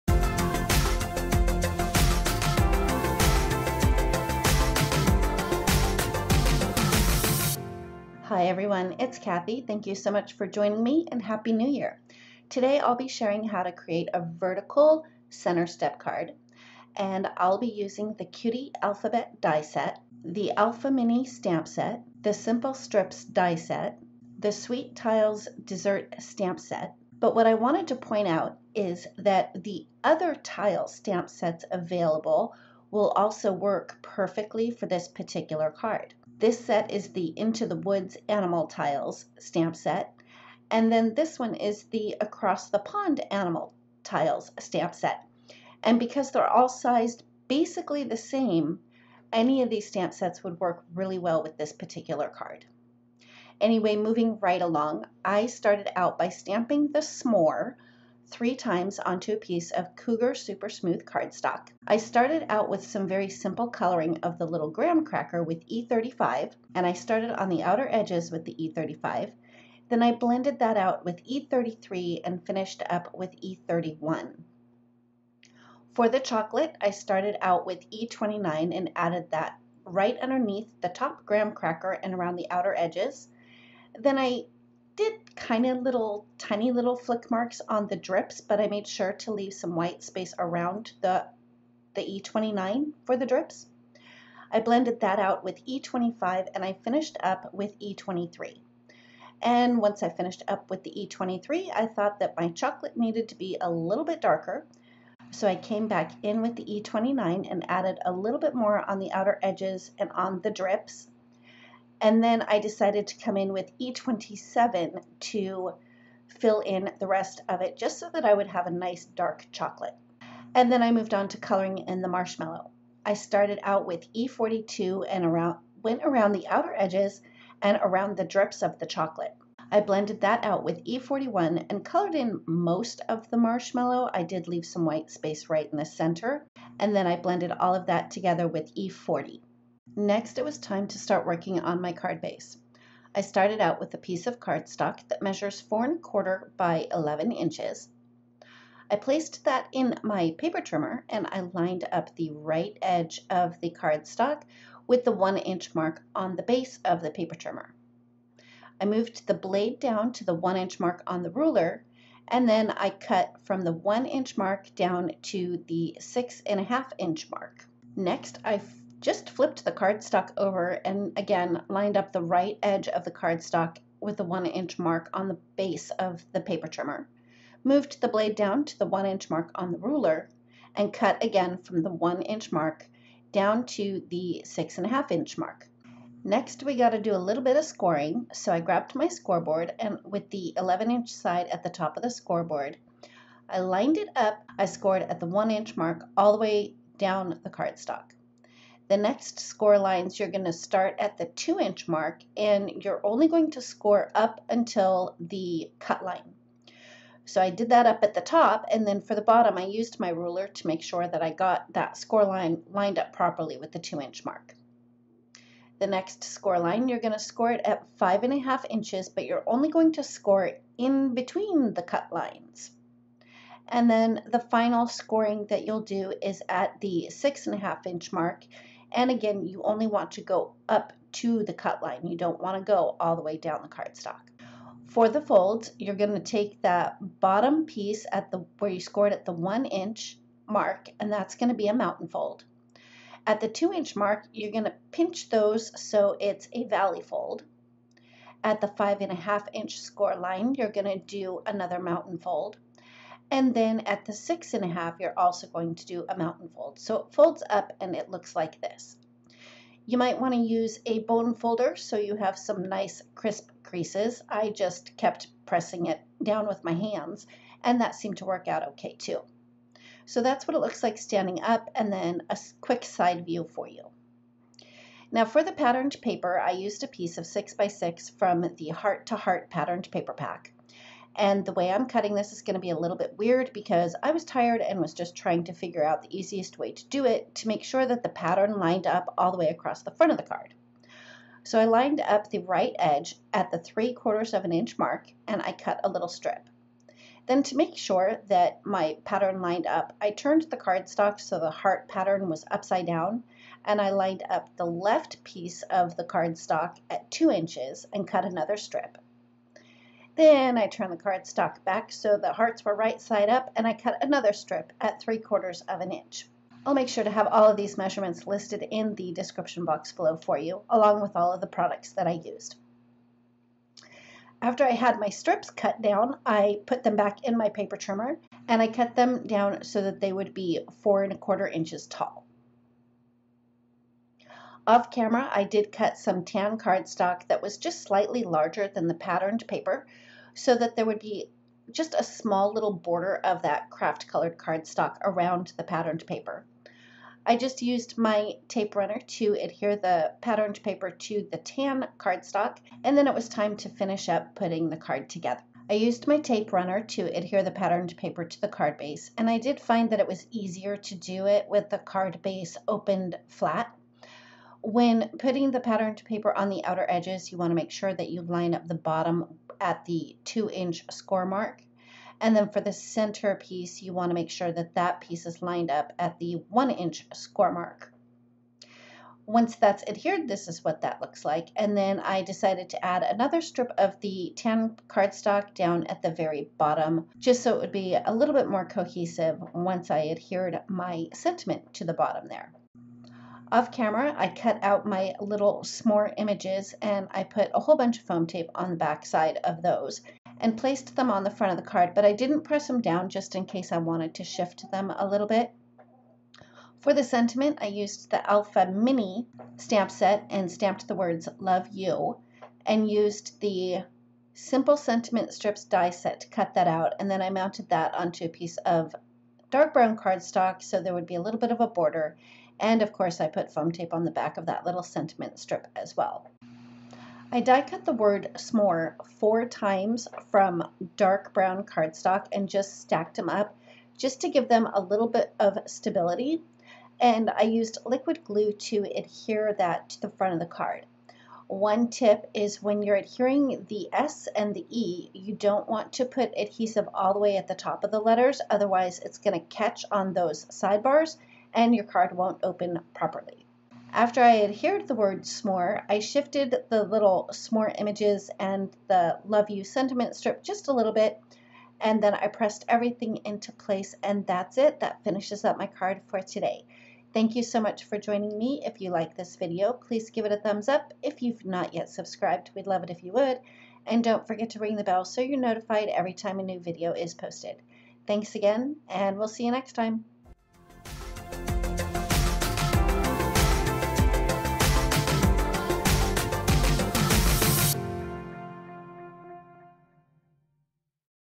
Vertical Center Step Card | A Not-So-Girly Valentine's Day Card
Oh, hi! Today I'm sharing how to create a Vertical Center Step Card featuring the Sweet Tiles Desserts Stamp Set.
CLICK SHOW MORE for product links! 👇🏻👇🏻👇🏻

***SUPPLIES USED***

Measurements:

Card base - 4.25" x 11" (10.8cm x 28cm)

Patterned Paper:
2 pcs - 0.75" x 4.25" (1.9cm x 10.8cm)
1 pc - 2" x 4.25" (5cm x 10.8cm)

Kraft Cardstock:
2 pcs - 7/8" x 4 3/8" (2.25cm x 0.95cm)
1 pc - 2 1/8" x 4 3/8" (0.42cm x 11cm)
1 pc - 4 1/8" x 7/8" (10.6cm x 2.25cm)

Cut from 1" to 6.5" (2.5cm to 16.5cm)

Score at 1" (2.5cm)
Score to the cut line at 2" (5cm)
Score at 5.5" in between cut lines (13.9cm)
Score to the cut line at 6.5" (16.5cm)

Copics Used:
Graham Cracker - E35, E33, E31
Chocolate - E29, E25, E23, E27
Marshmallow - E42, E41, E40

Favorite Tools

Brother Scan N' Cut

Gemini Jr. Die-Cutting Machine

Make-Up Brushes for Ink Blending